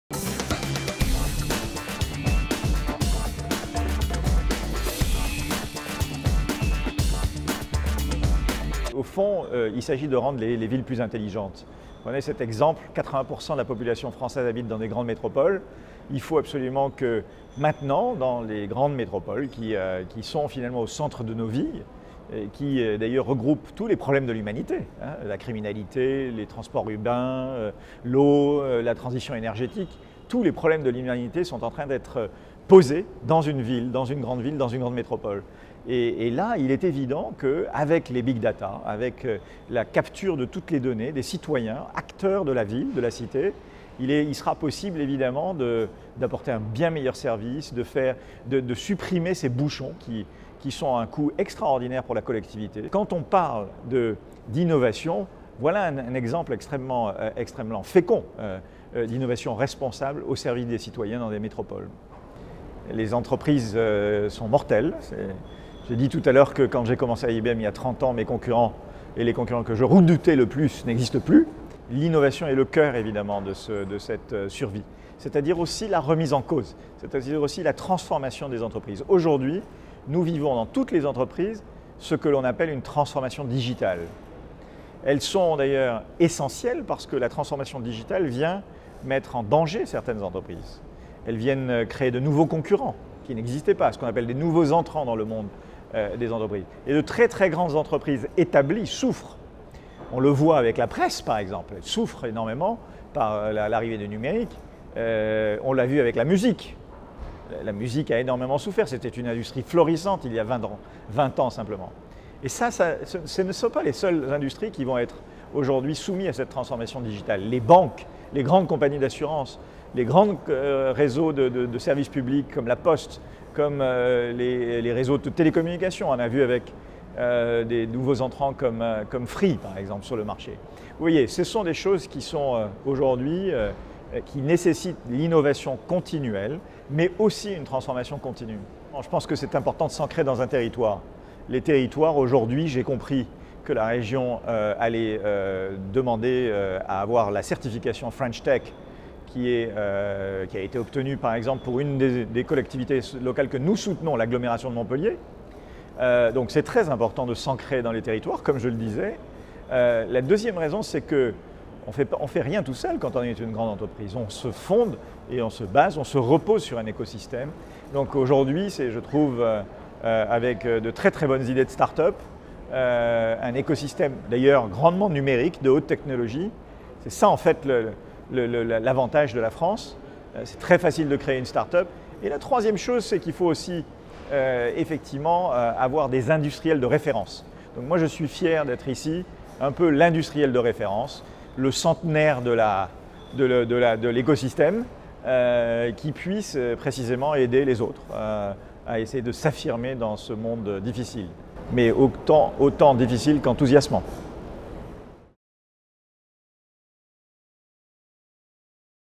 Alain Bénichou, Président IBM France - Innovation Responsable en Lorraine
Témoignage recueilli lors de la journée "Osez l'économie de demain" le 20 novembre 2014 à Nancy.

En savoir plus :
Evénement phare de la démarche « Innovation Responsable en Lorraine » pour l'année 2014, le Groupe La Poste et son cercle de partenaires organisent au Centre des congrès de Nancy, ce jeudi 20 novembre, les rencontres nationales : « Osez l'économie de demain : numérique, circulaire et solidaire ! »

Cet événement met en lumière notamment les nouveaux business models, l’énergie circulaire, les valeurs humaines au cœur de la nouvelle économie, mais aussi les parcours gagnants des entrepreneurs innovants français.

La journée est conçue sous forme d’incubateur éphémère avec quatre pôles de développement associés : un pôle « Conférences » animé par Laura Tenoudji, journaliste, et David Abiker, candide ; un pôle « Financement » pour concrétiser ses projets ; un pôle « Open Innovation » au sein duquel des ateliers et démos d’innovation sont proposés, un pôle « Solutions business » organisé dans le village des partenaires…

Parmi les intervenants aux conférences figurent notamment Erik Orsenna, économiste et écrivain, Alain Bénichou, Président IBM France, Stéphane Quéré, Directeur Innovation de GDF SUEZ, Sophie-Noêlle Némo, Directrice Délégation à la Transition énergétique du Groupe La Poste, Hugues Hansen, Directeur général de Start’inPost ou encore Marc-David Choukroun, co-fondateur de La ruche qui dit Oui !